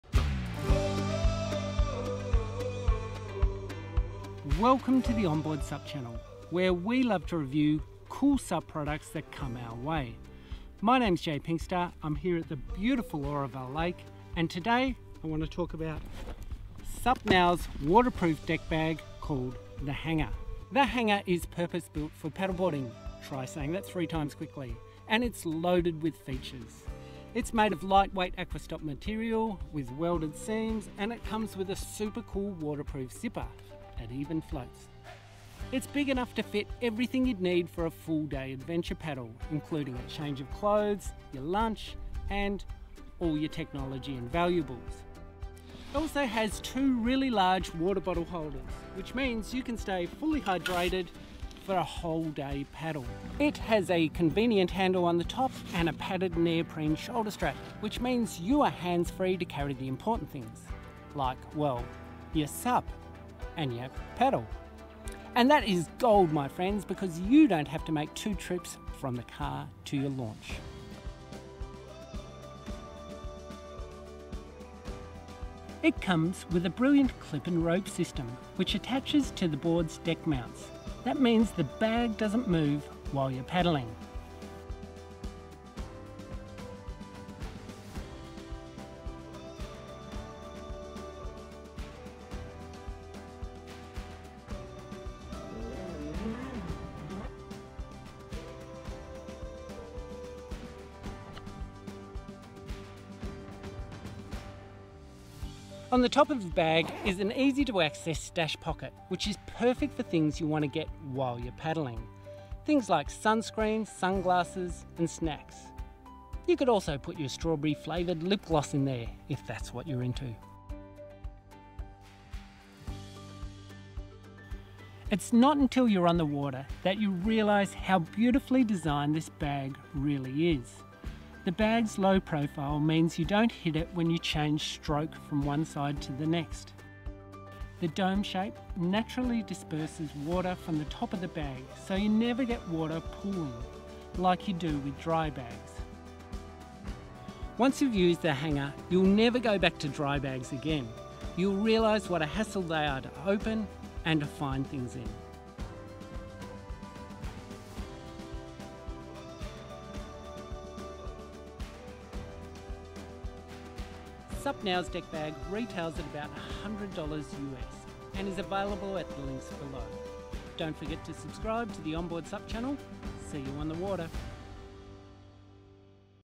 The Hangar
The Hangar is one the best solutions that I've come across for waterproof storage on your SUP. It's well designed and loaded with features. Check it out.